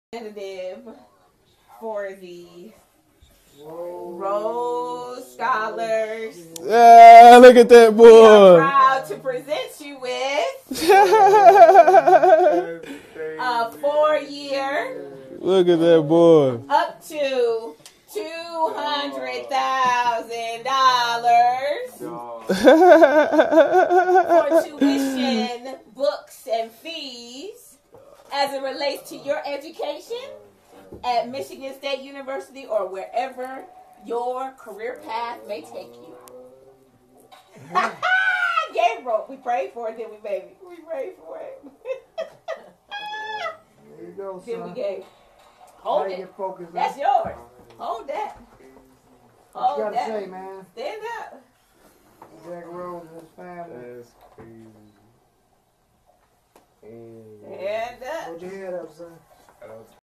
D Rose and his organization surprised him with a $200,000 scholarship🙏
D Rose and his organization surprised him with a $200,000 scholarship to attend Michigan State University. 🙏